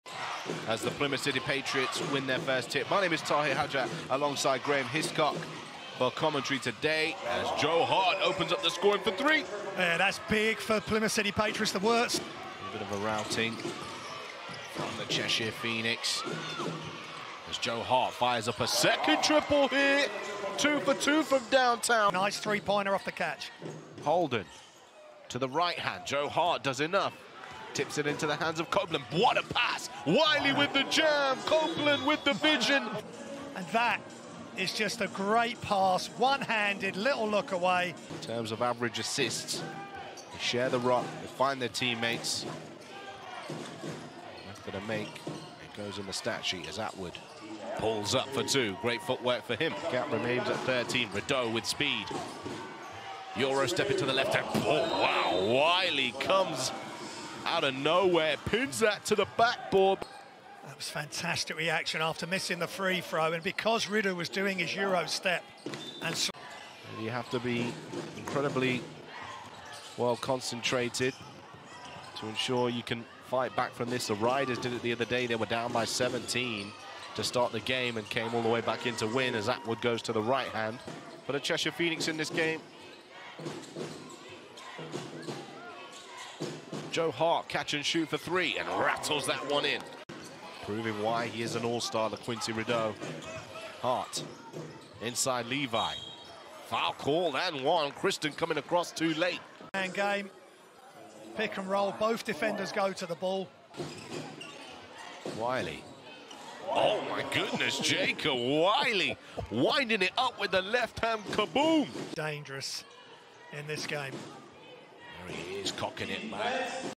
Plymouth City Patriots Highlights vs. Cheshire Phoenix
Plymouth City Patriots Highlights vs. Cheshire Phoenix, 03/09/2024

00:00:00 - 1st Quarter
00:00:22 - 2nd Quarter
00:00:38 - 3rd Quarter
00:01:44 - 4th Quarter